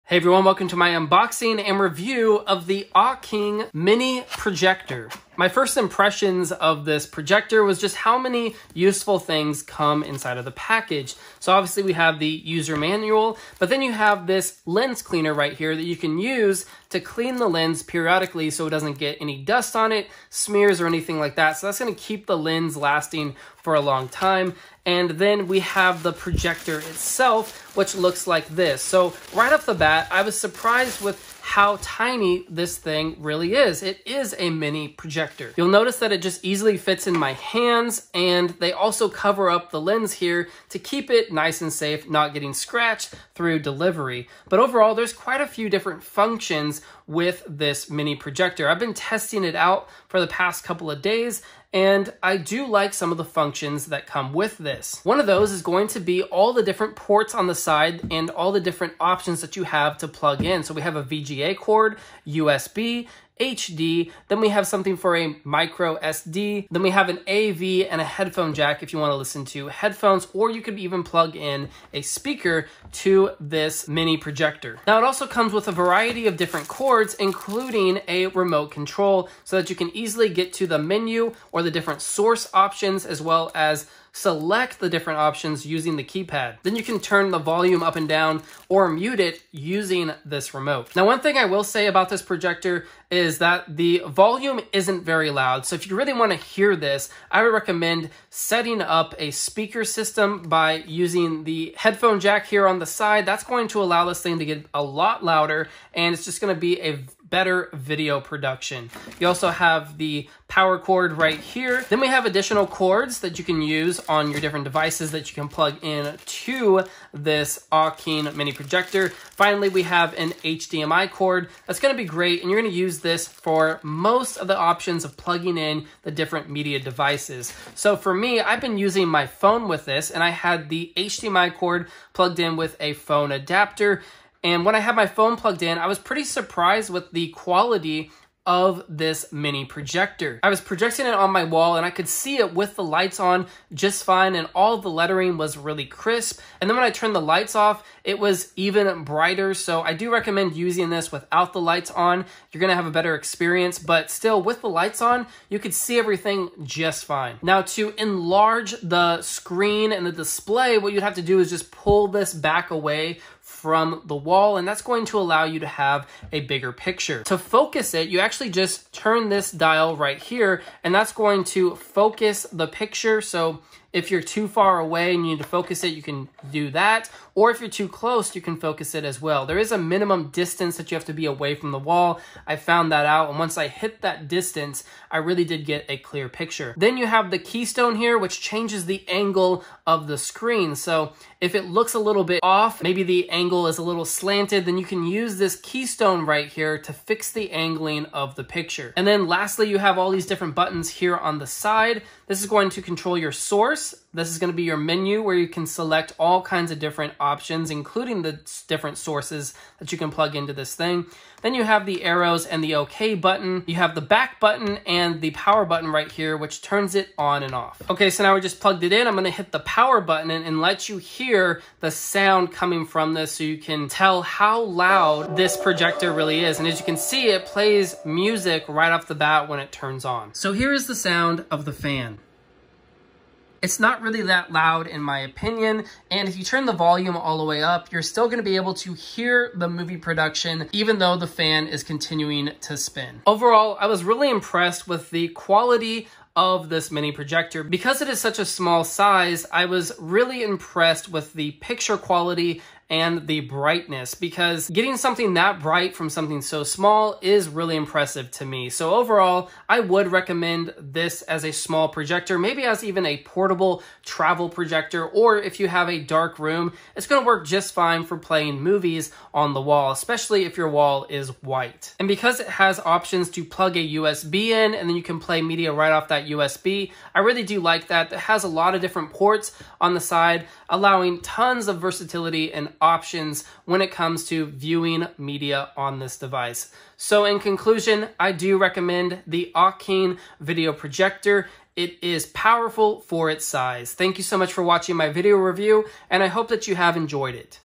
AuKing Projector Review: Best Selling $90 Mini Projector Worth It?
AuKing Projector
Check On Amazon

The AuKing Projector is a 1080P min i projector selling for just $90.
The AuKing Projector has been a best selling projector on Amazon for awhile.
I personally think the AuKing Projector is a great budget projector.
Is the AuKing Projector the best budget projector you can buy?
This video will be my honest review and unboxing of the AuKing Projector mini projector. By the end of the video you will know whether or not the AuKing Projector is worth it in 2023.